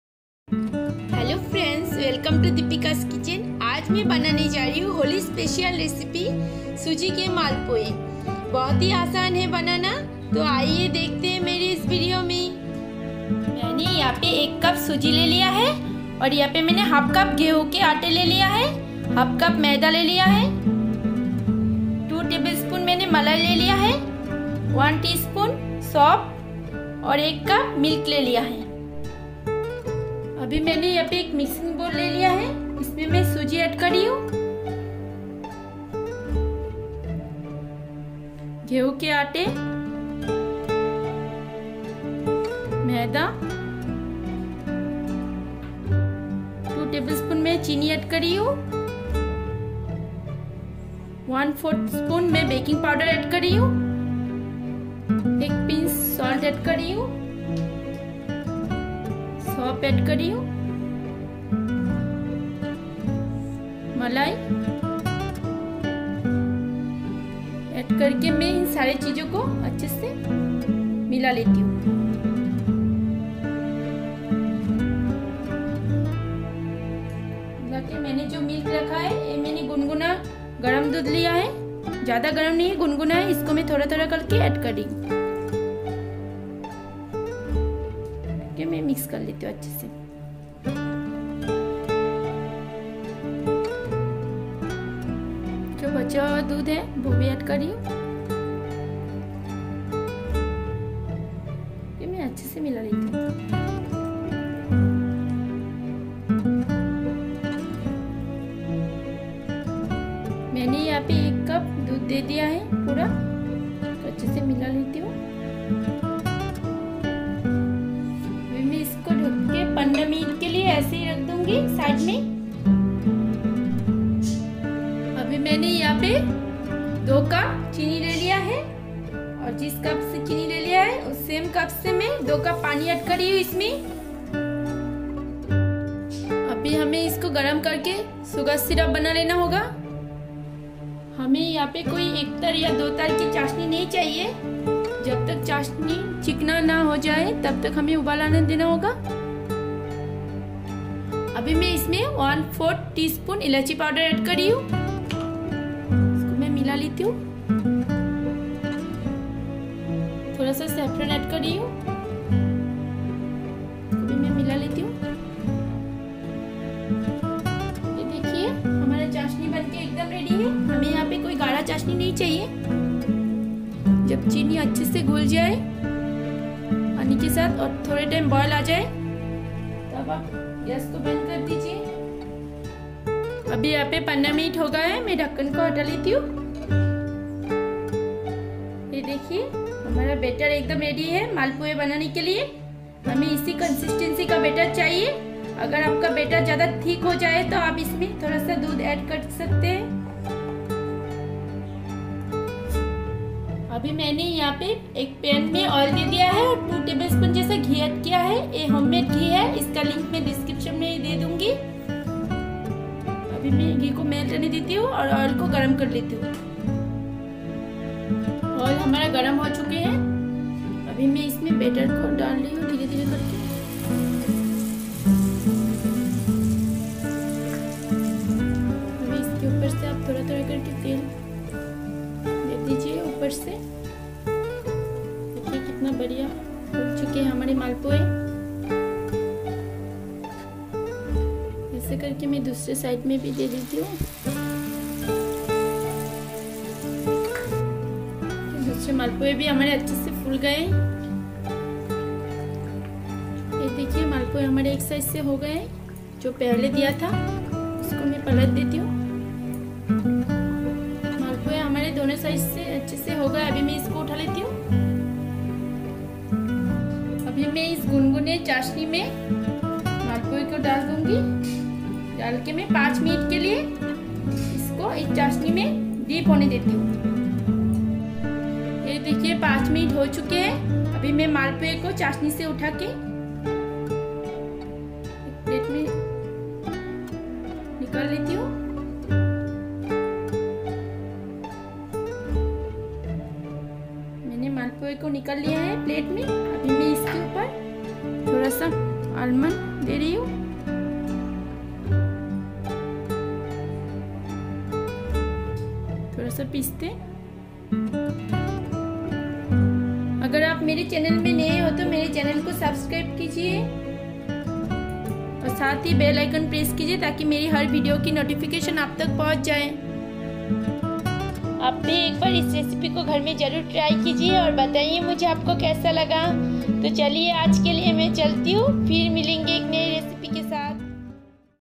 Suji Ke Malpua | Holi Special Recipe Suji Malpua | मालपुआ बनाने का सबसे आसान तरीका | Dipikaskitchen
Suji Ke Malpua Recipe @DipikasKitchenoriginal
Ingredients :

Suji : 1 cup
wheat : 1/2 cup
Flour : 1/2 cup
Milk : 1 cup
Milk cream - 2 tbsp
Backing powder - 1/4 tsp
Oil
Dryfruits